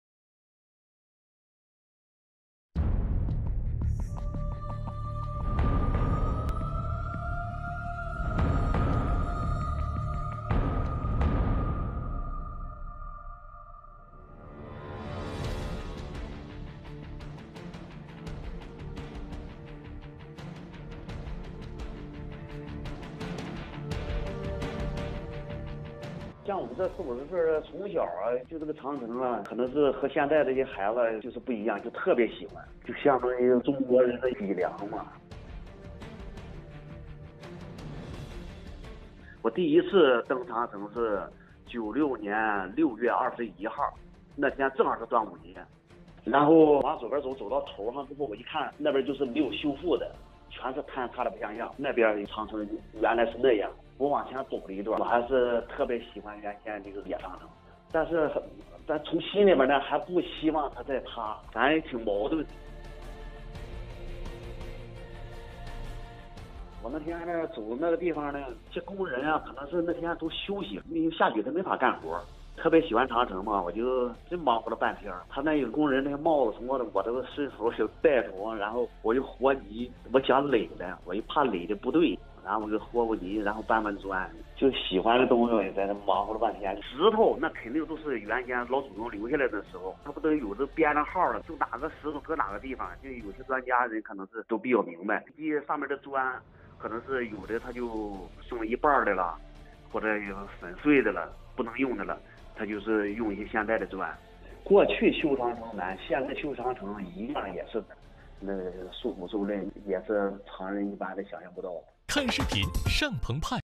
45-year-old man shows deep love to Chinese backbone 'Great Wall' in Beijing
A 45-year-old man gets obsessed with the GreatWall after visiting there 23 yrs ago. He comes across a restoration team & volunteers to help the workers mix sand & cement together. The Great Wall symbolizes the Chinese backbone, he says. (Video: Kuaishou)